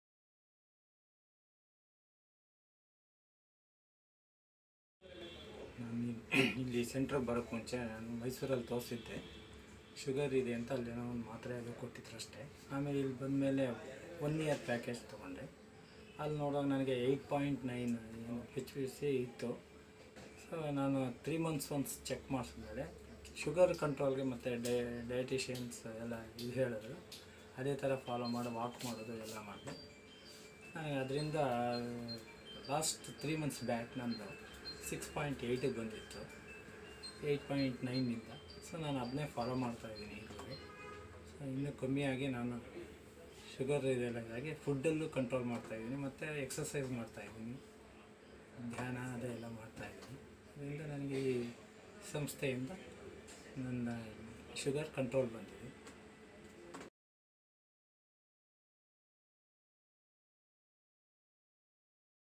Testimonial from Diabetes Patient at Padmanabhanagar
A drop of HbA1c from 8.9 to 6.8! Moving towards diabetes control…Apollo Sugar success story